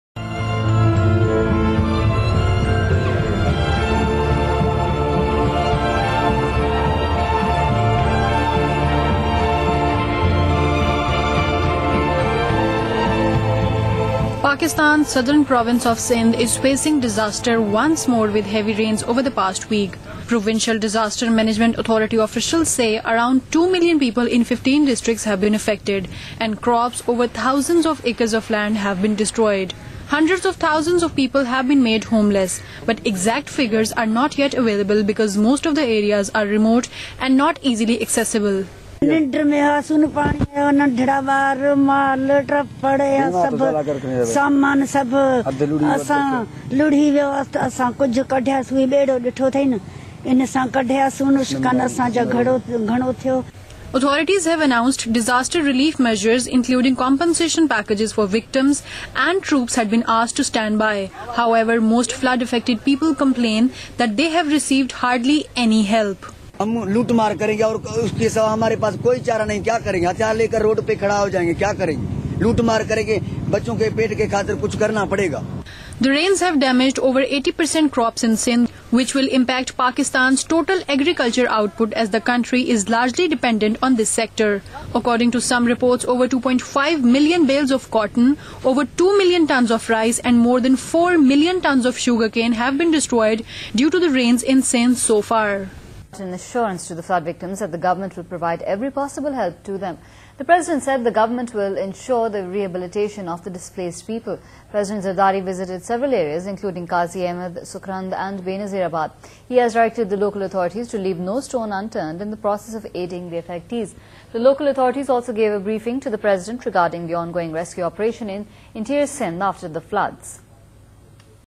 President pays heed to pleas, visits flood affected areas
Floods ravage Sindh another day, while authorities reportedly are providing relief effort to the affectees along with compensation.
President Zardari visited the affected area of Benazirabad on Wednesday, meeting with concerned authorities along with CM Qaim ali Shah and MNA Faryal Talpur. He directed the CM to personally monitor the situation and relief efforts.